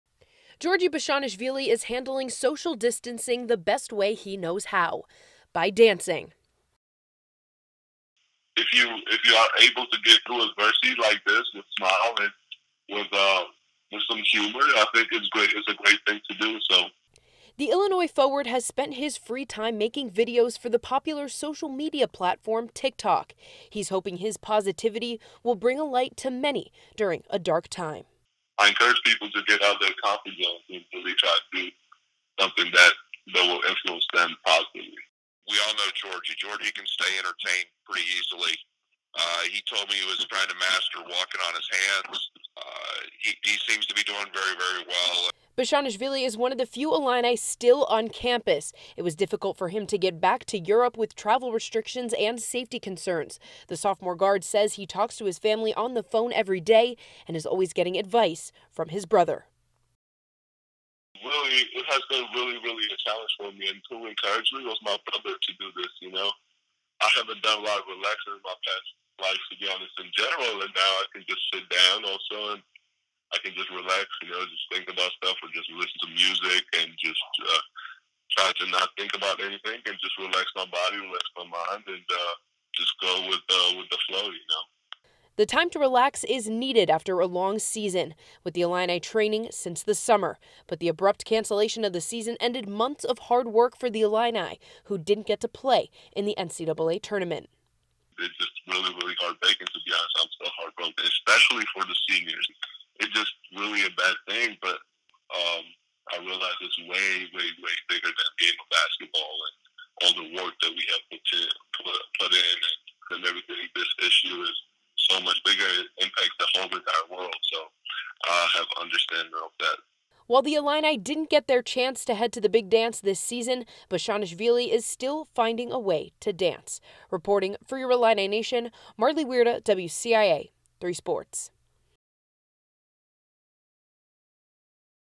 Giorgi Bezhanishvili shares positivity amid global pandemic
CHAMPAIGN (WCIA) -- Giorgi Bezhanishvili is handling social distancing the best way he knows how—by dancing. The Illinois forward has spent his free time making videos for the popular social media platform TikTok. He’s hoping his positivity will bring a light to many during a dark time.